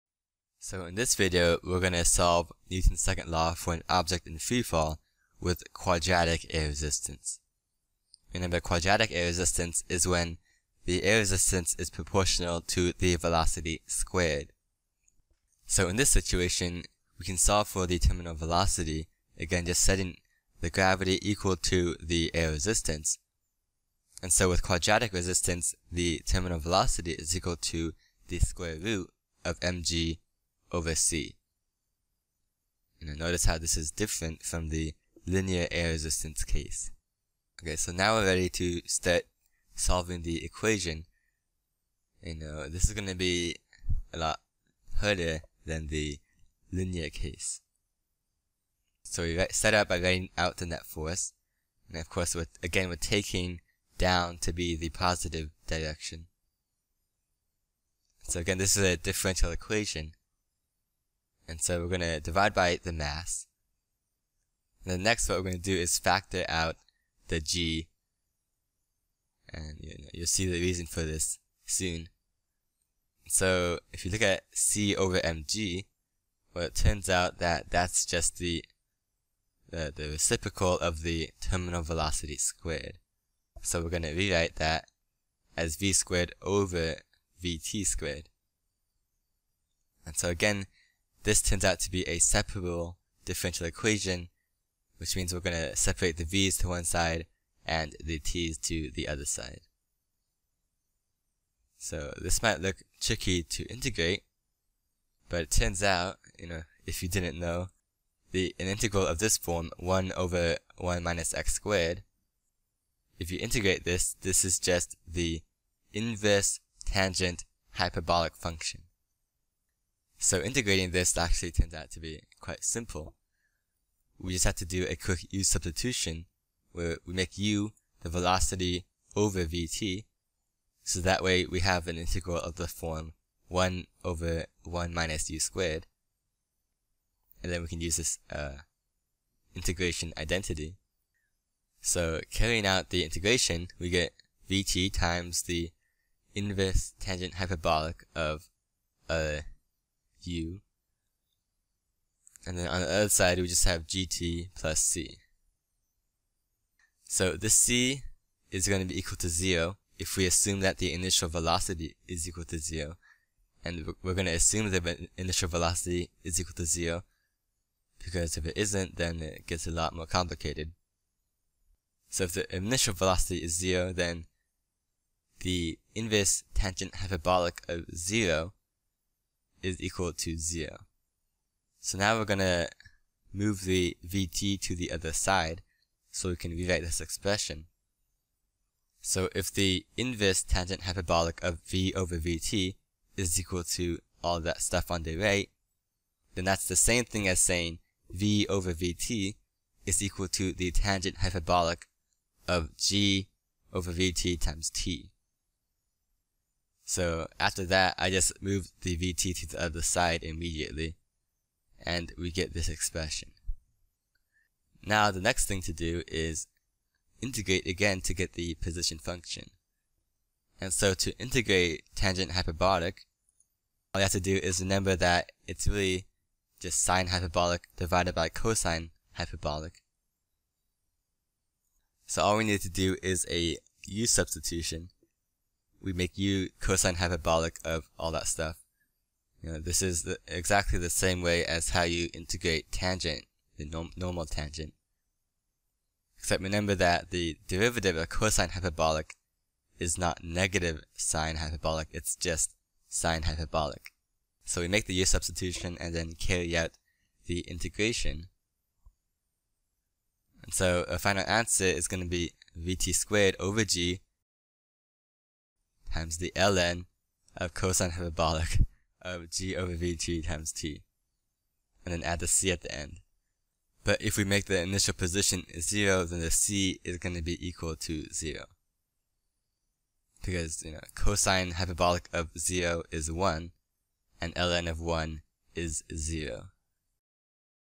[Physics] Free Fall With Quadratic Air Resistance
In this video I solve Newton's Second Law for free fall with quadratic air resistance.